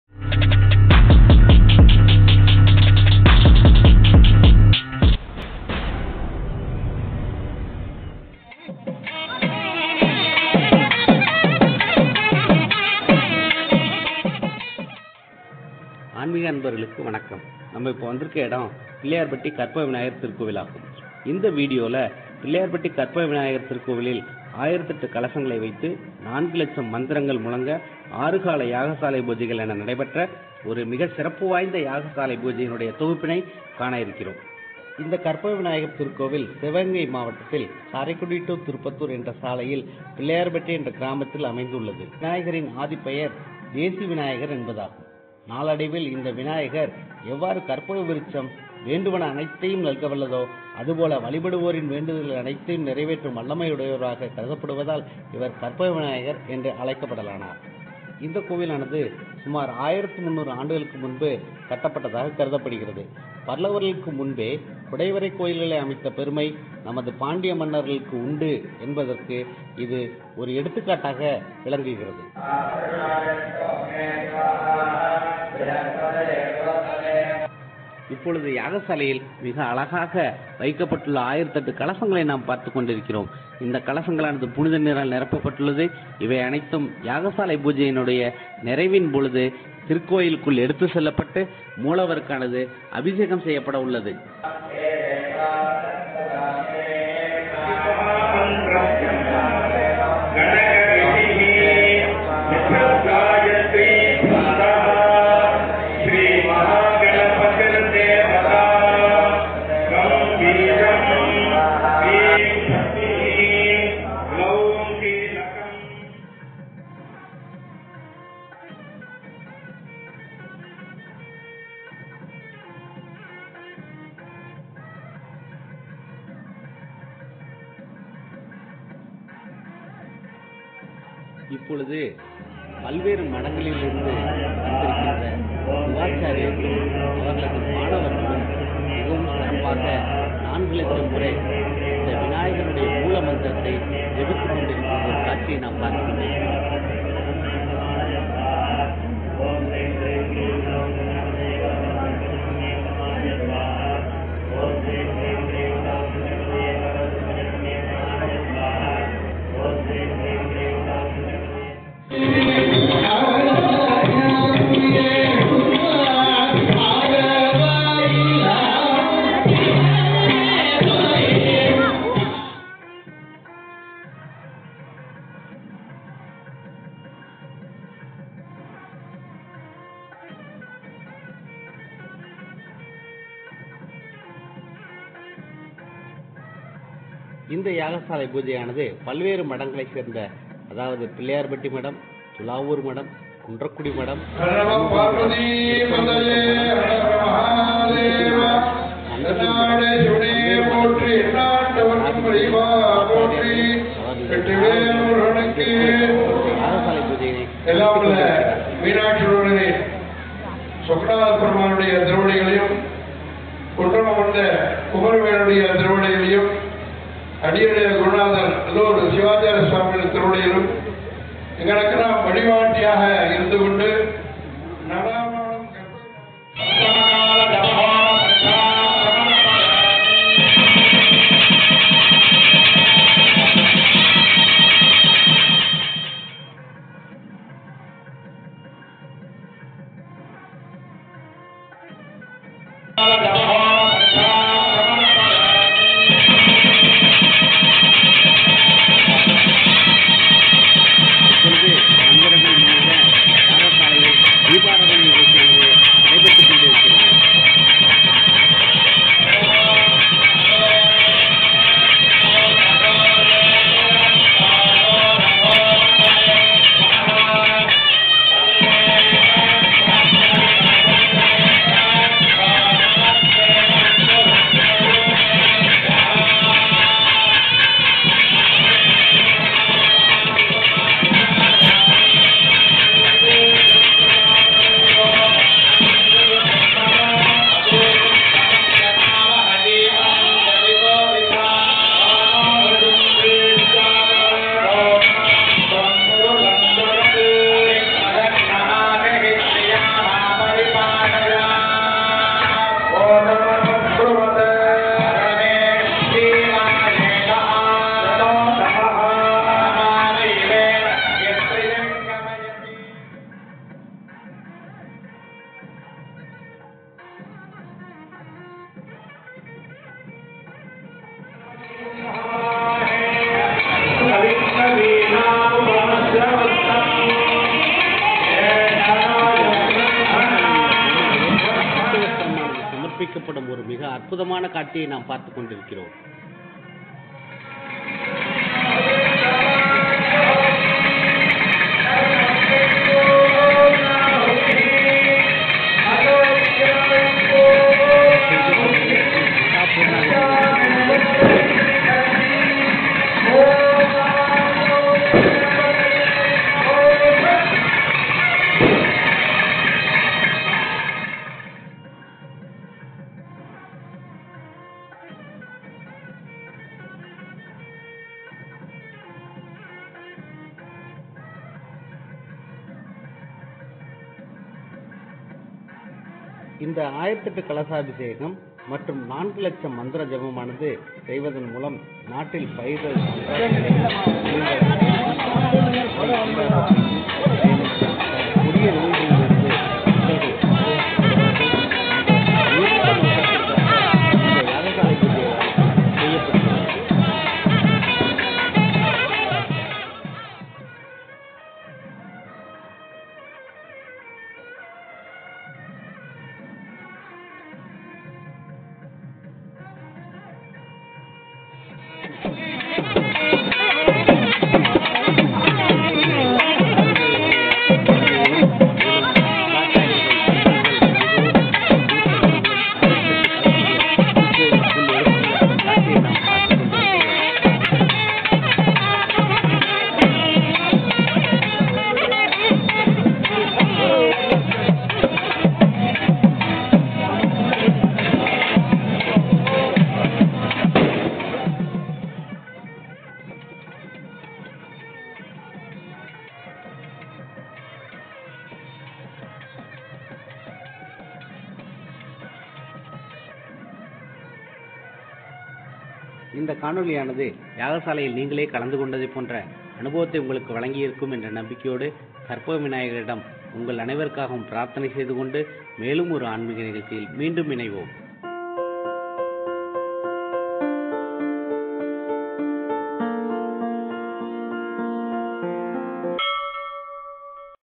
1008 கலசாபிஷேகம் | கற்பக விநாயகர் திருக்கோவில் , பிள்ளையார்பட்டி ( 4 லட்சம் மூல மந்திர ஜெபம் )
அருள்மிகு பிள்ளையார்பட்டி கற்பகவிநாயகர் திருக்கோவிலில், 1008 கலசாபிஷேகம் மற்றும் 4 லட்சம்  தடவை மூல மந்திர ஜெபம் மற்றும் 6 கால யாக சாலை பூஜைகள் சிறப்பாக நடைபெற்றது.  இந்த திருவிழாவானது உலக நலன் வேண்டி பல்வேறு மடங்களை சார்ந்த சிவாச்சாரியார்கள் , அவரது மாணவர்கள் கலந்து கொள்ள  சிவ ஆகம விதிகளின் படி நடந்தது. அந்த நிகழ்ச்சிகளின் சில காட்சிகளை உங்களுக்கு வழங்குவதில் நங்கள் பெருமை கொள்கிறோம்.  யாம் பெற்ற இன்பம் பெறுக இவ்வையகம்.